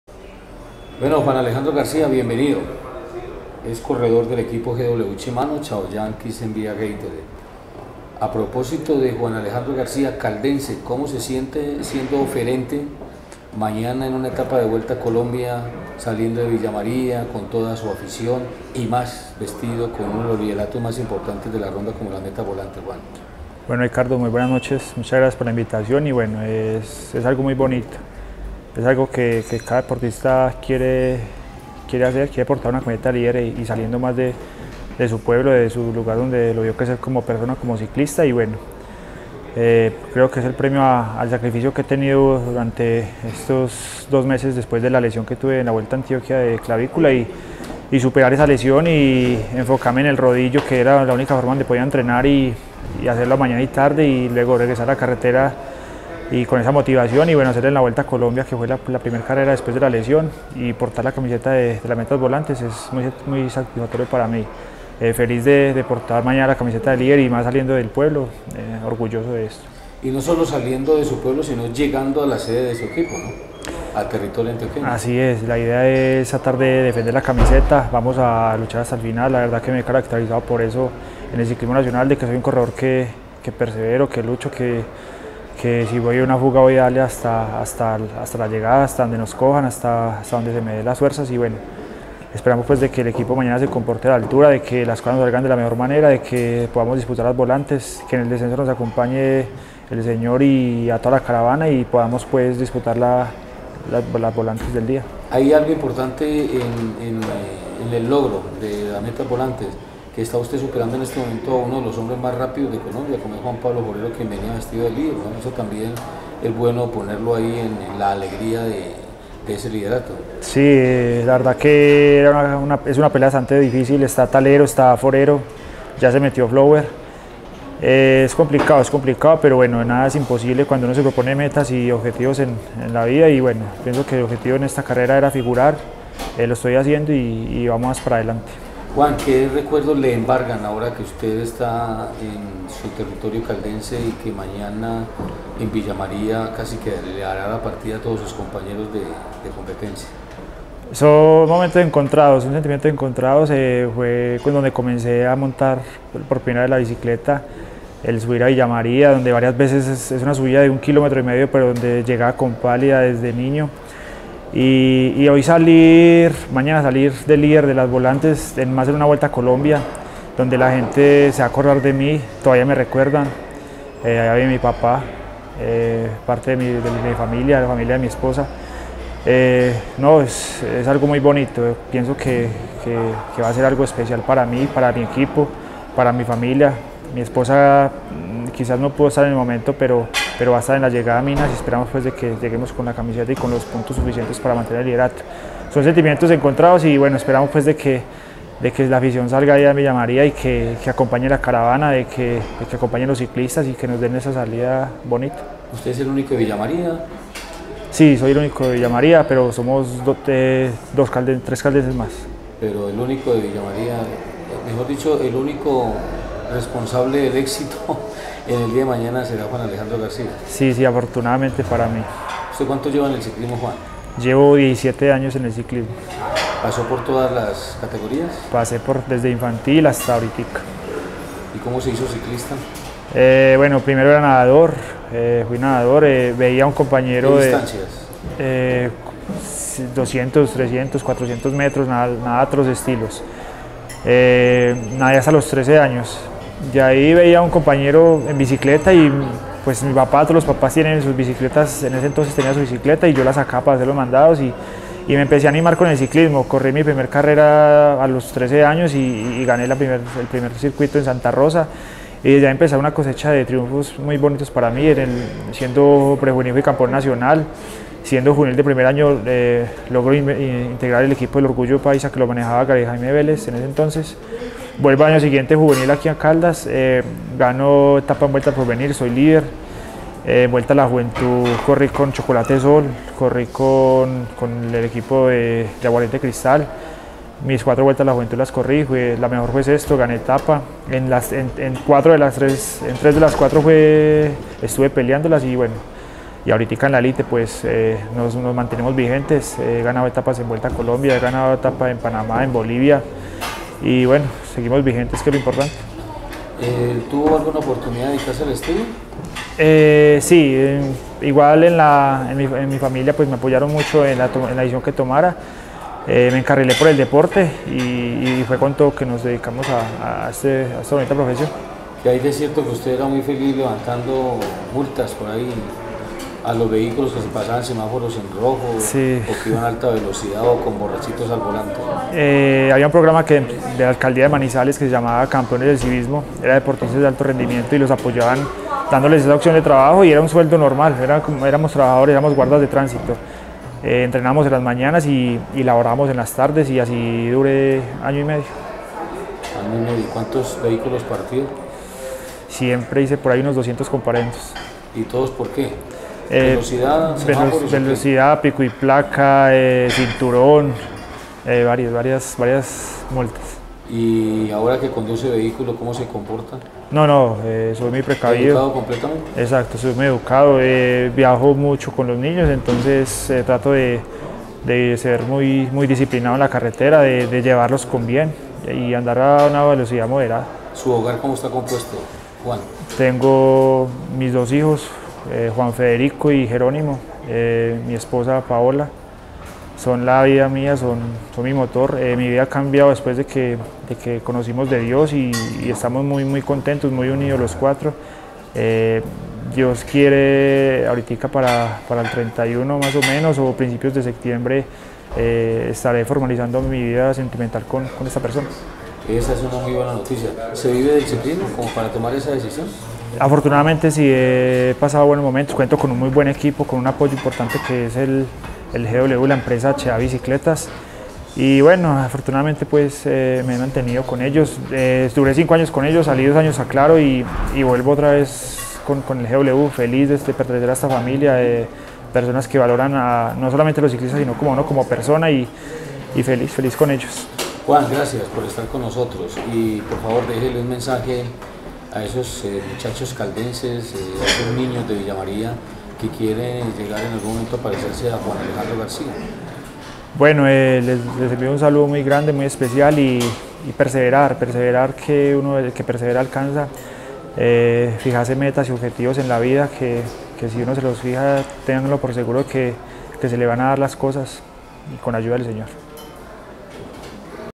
Juan Alejandro Garcia - Ciclista
Conozca un poquito más del AtletaGW Juan Alejandro García, en entrevista con Aicardo Torres.